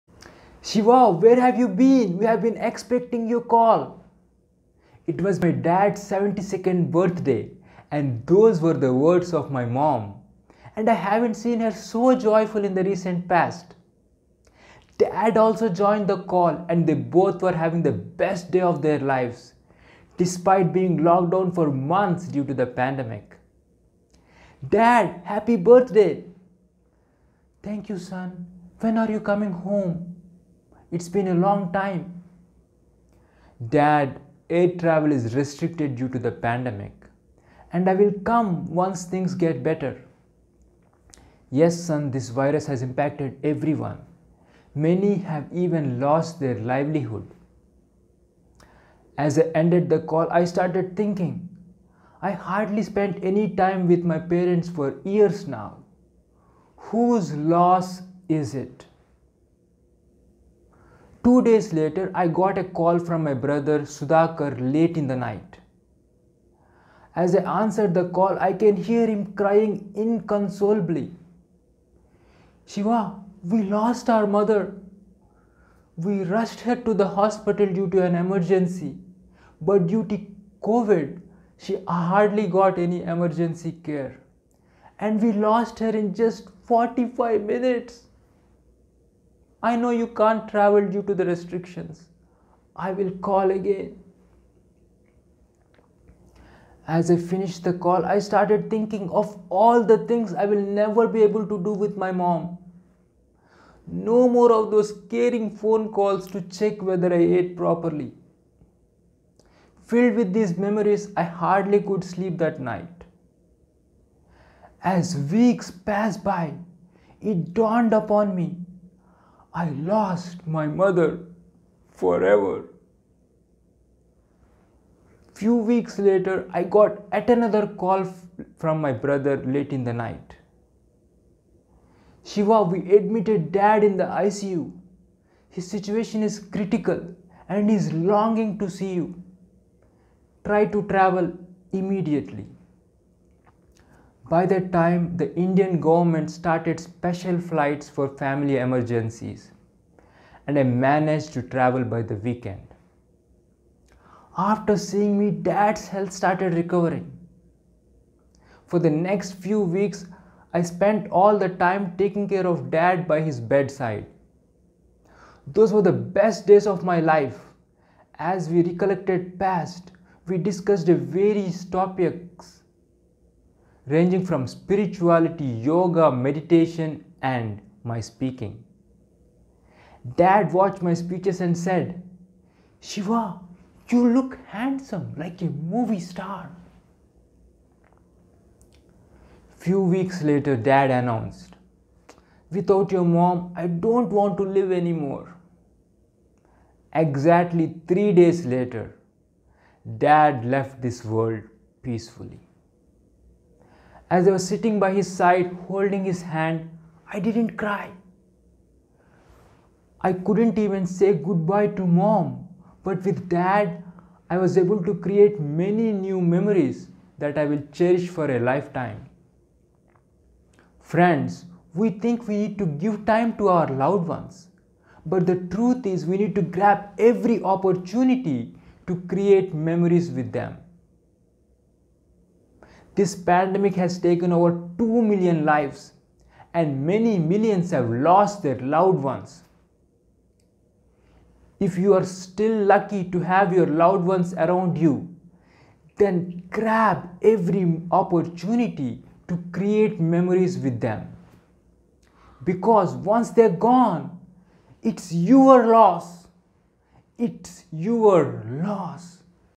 When Love is Lost - Shiva Duddi
Love lessons contestant at Speaker Slam

When it comes to our loved ones we most often think we need to give time to them but we never realize their importance until we loose them. The pandemic took away more than 2 million lives and many millions have lost their loved ones.
Its important to understand the importance of spending time with our loved ones when they are around.